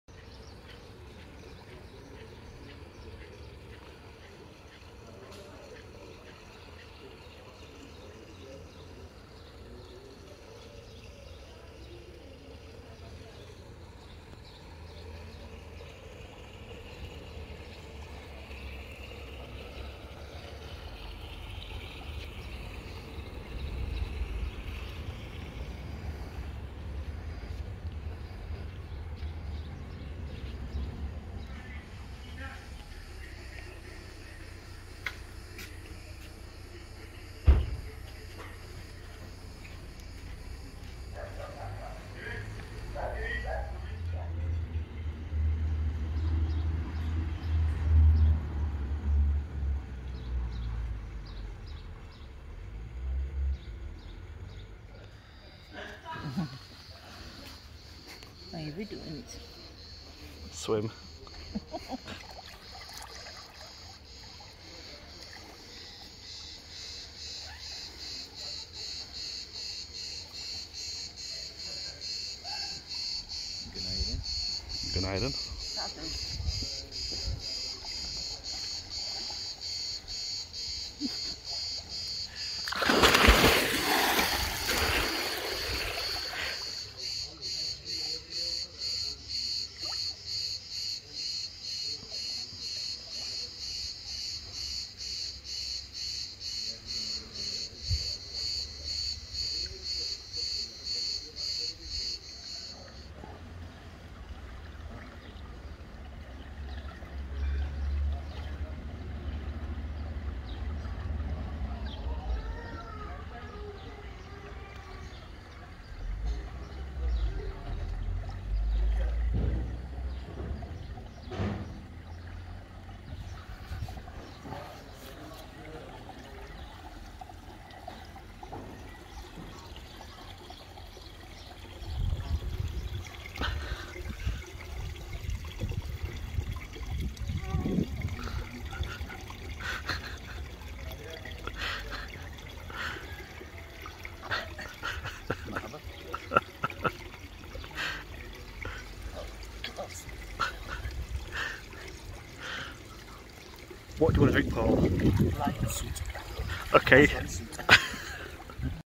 The Gorkem Hotel Ovacik Turkey | July 2022
The lovely Gorkem Hotel. Amazing Freindly staff and great food. And lovely pool